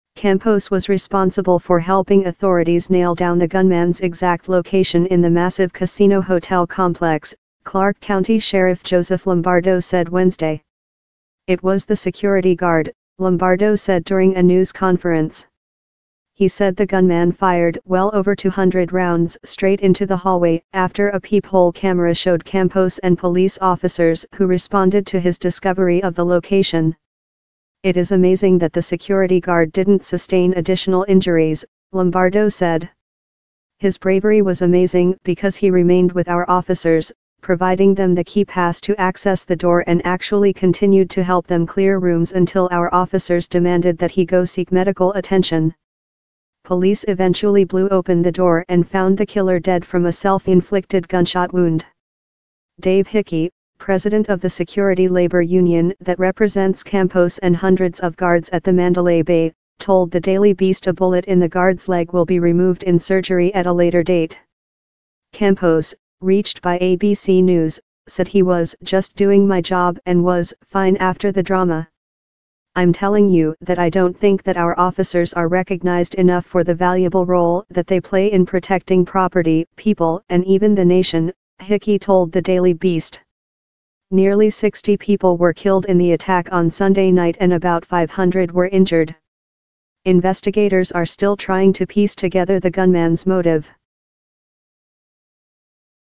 Unarmed Hotel Security Guard Who Found Las Vegas Shooter Hailed As Hero
Campos was responsible for helping authorities nail down the gunman exact location in the massive casino-hotel complex, Clark County Sheriff Joseph Lombardo said Wednesday.